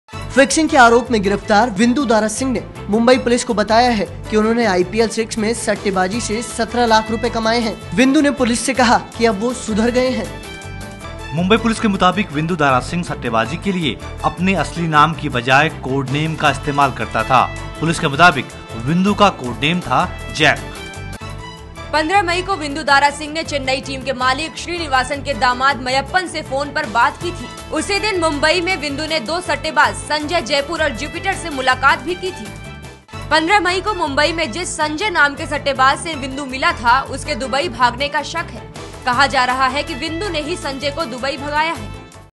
Spot fixing: Vindoo used code name for betting
Vindoo Dara Singh used to use code name for betting in IPL and he earned 17 lakh by betting in sixth edition of IPL.